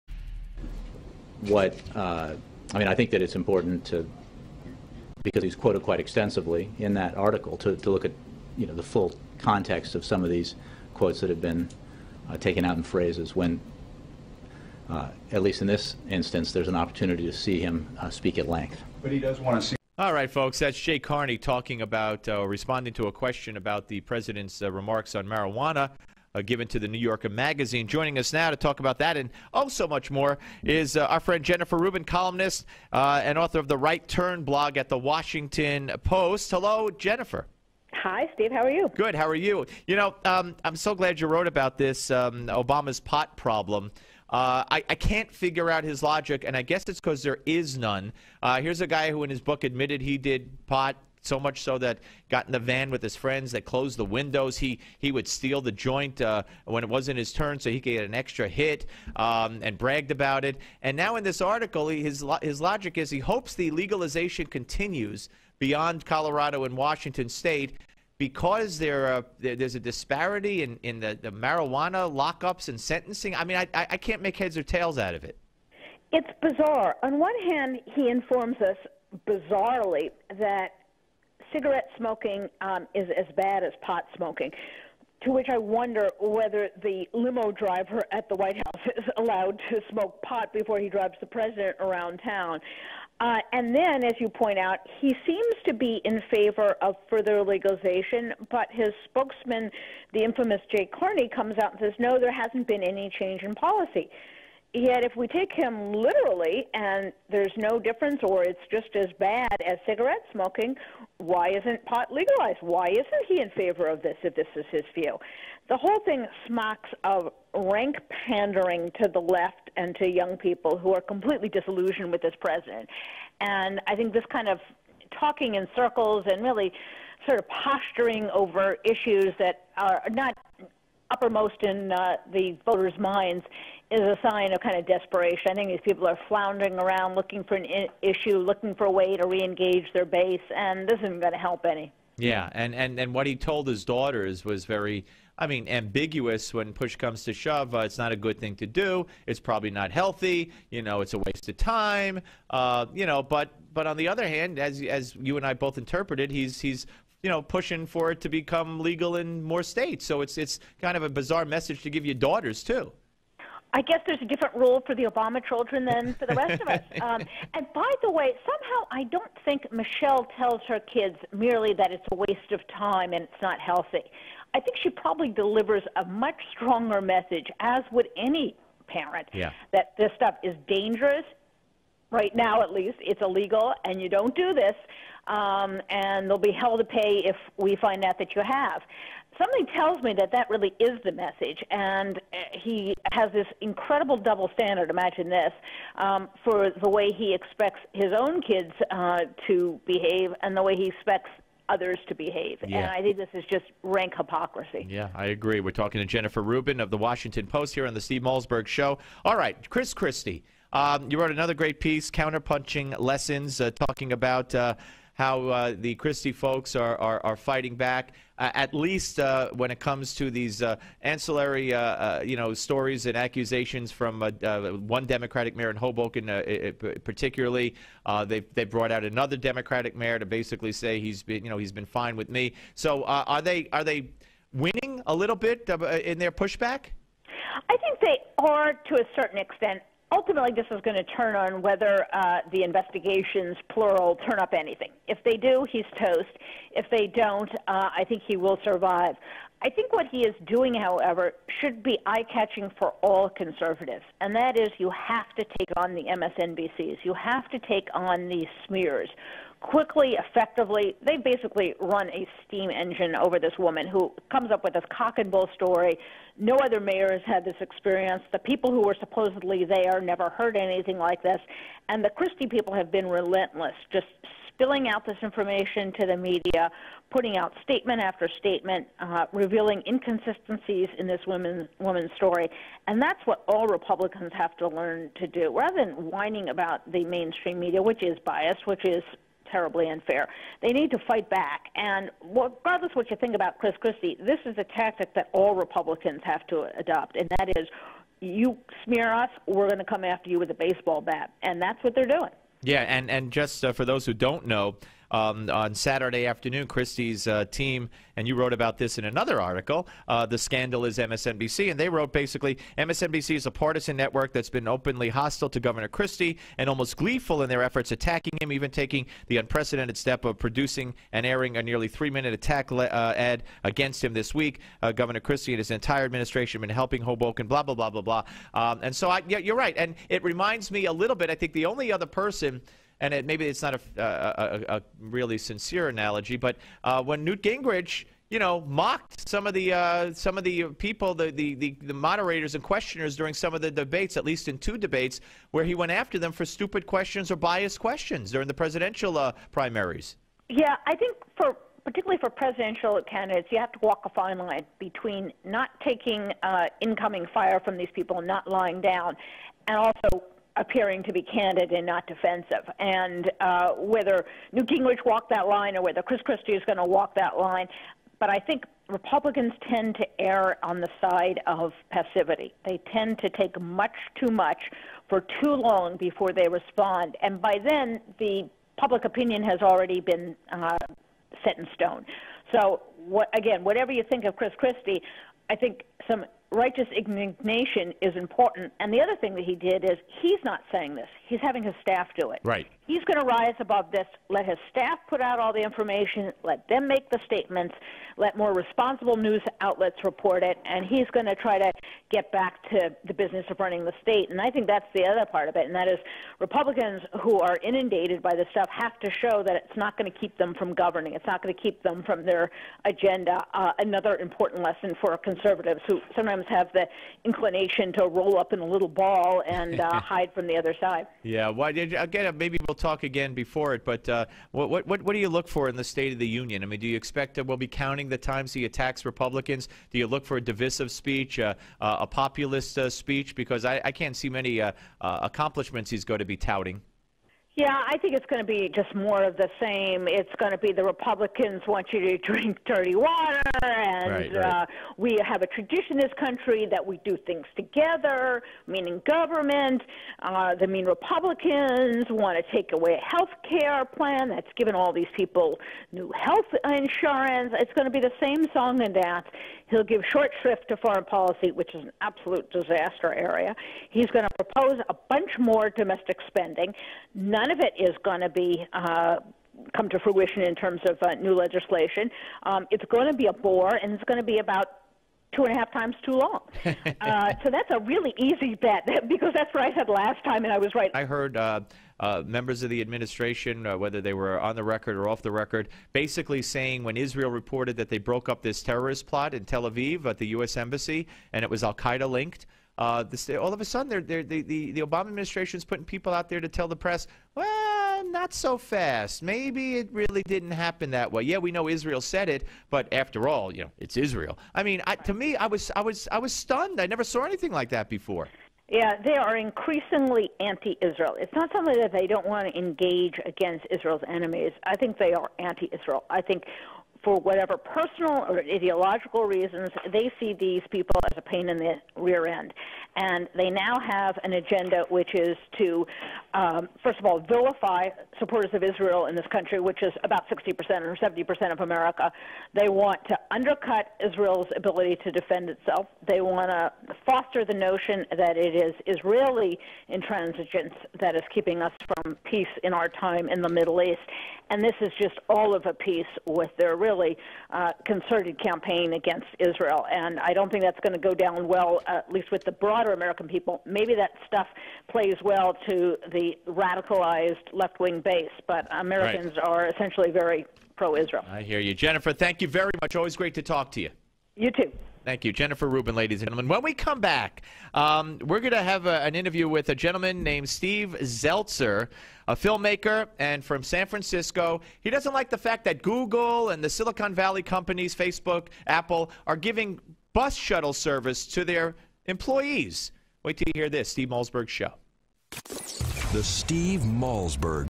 Jennifer Rubin - Columnist and author of the "Right Turn" Blog at The Washington Post
joins Steve to condemn MSNBC and praise Chris Christie